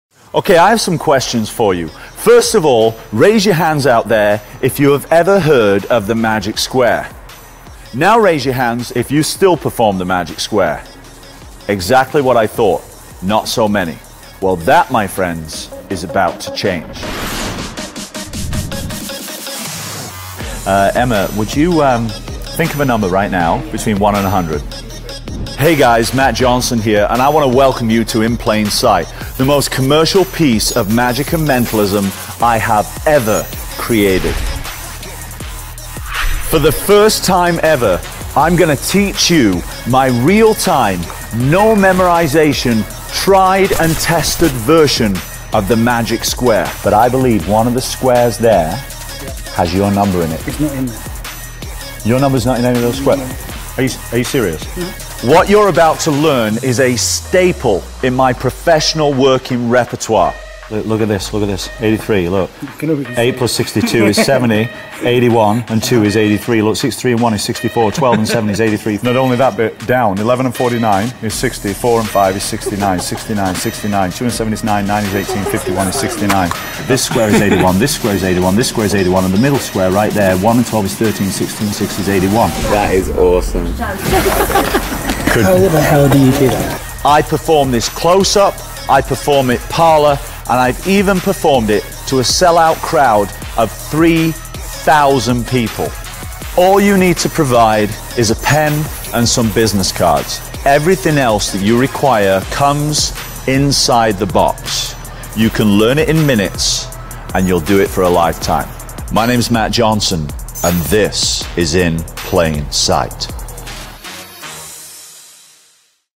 In Plain Sight by Matt Johnson
In Plain Sight (Gimmick and Online Instructions) by Matt Johnson! Have you wanted to perform The Magic Square but have been afraid to do so? Problem solved!

The Magic Square - an extraordinary piece of magic and mentalism that layers the improbable over the impossible. For Britain's Got Talent sensation and long-time beard aficionado, Matt Johnson, The Magic Square was nearly perfect, except for the amount of intense focus and memory work that was required to perform the effect.

Now Matt Johnson delivers a version that strips away all of that memory work and sets this powerful classic on easy mode! Never again strain to remember the secret numbers. Never again attempt mental gymnastics while you perform. Never again perform The Magic Square without Matt Johnson's In Plain Sight.